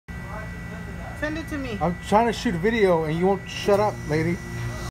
How to talk to Managers at work
Manager ruining an epic short film. The name of the short was “Famine In Front of Feast”. It was supposed to be short glimpses into the predicament a cockroach finds its self fucking dying in front of a tune of food. I don’t think I can selvage the original concept of the film. So the next best thing to do is to take the footage an show the best way to address your manager/supervisors.

What really keeps me up at night is knowing that the initial film idea could have gotten some real attention in the short independent film festival. The unapologetic, single shot, raw, look into the heart breaking death of a young disgusting roach, would have forced the audience to reevaluate what is going on is today geo-world unrest.

But instead my hand was forced to adapt and refocus. All thanks to someone’s selfishness and inpatient. Maybe be that will what people will take away from this short; shut the fuck up when someone is filming